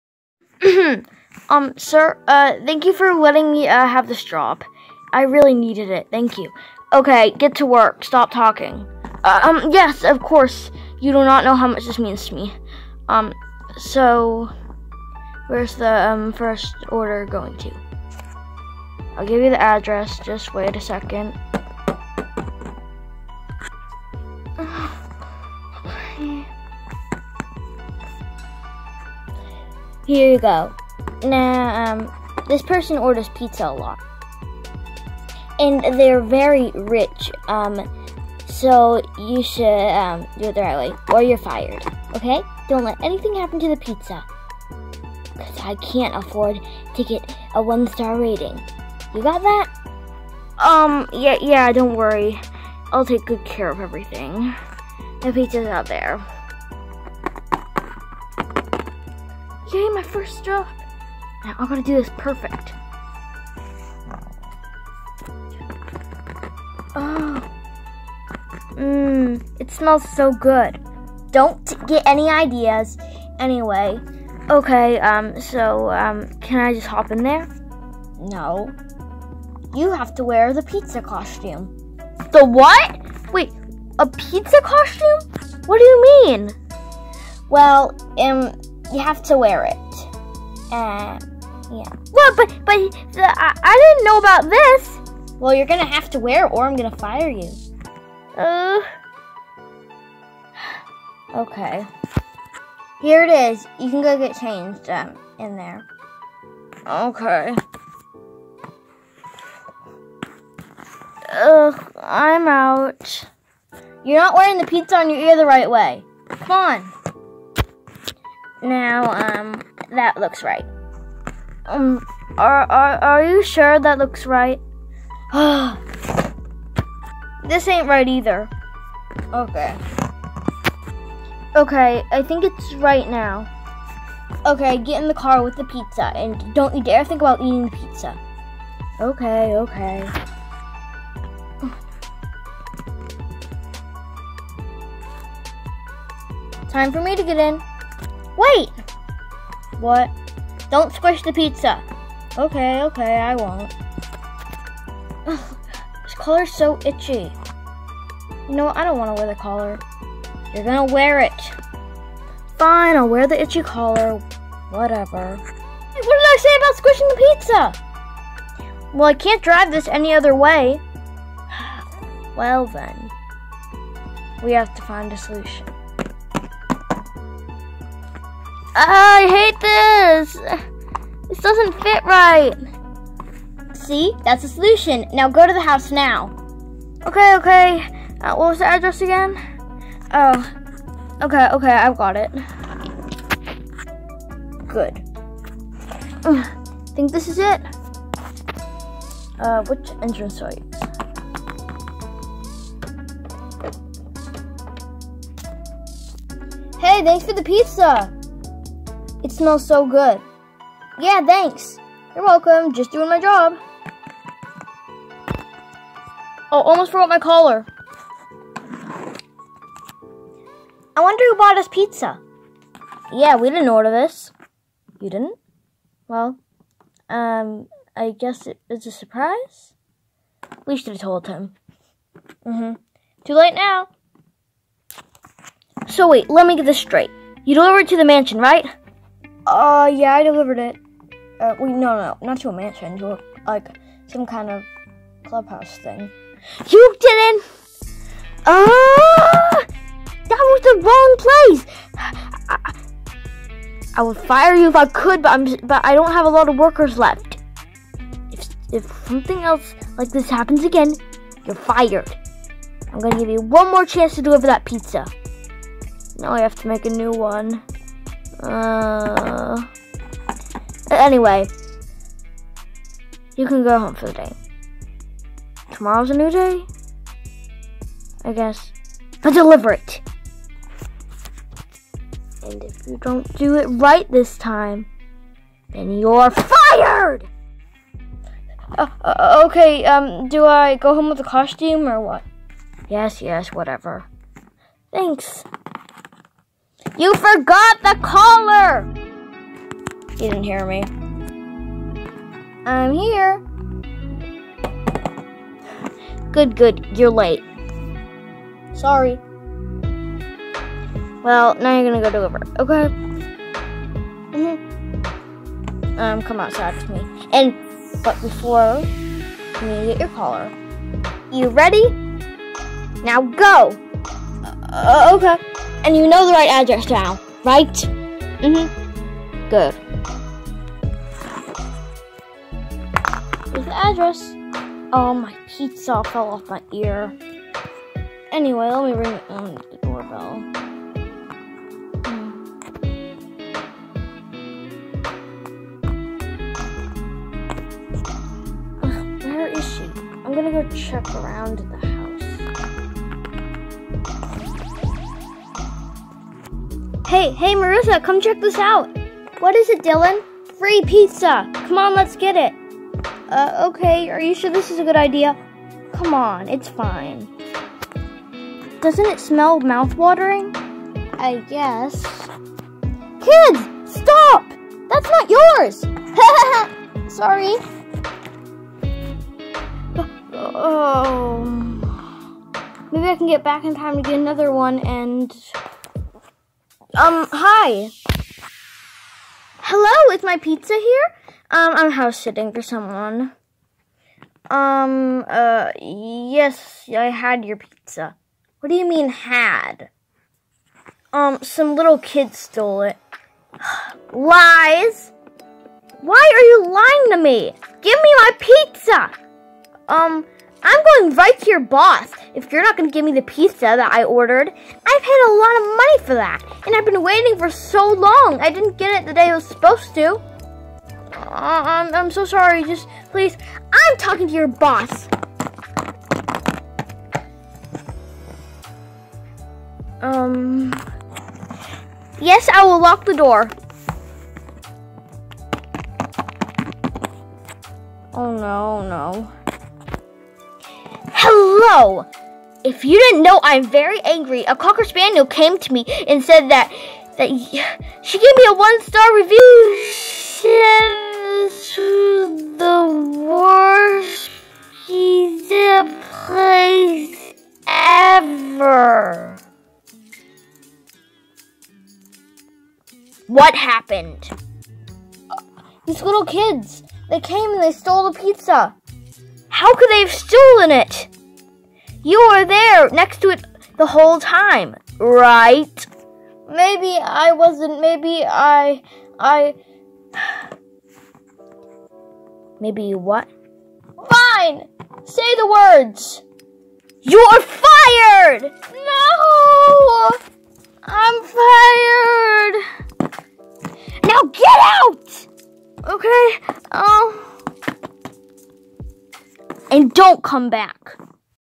lps: stolen pizza (short film)
longer version of "pizza thief"

all music in this video does not belong to me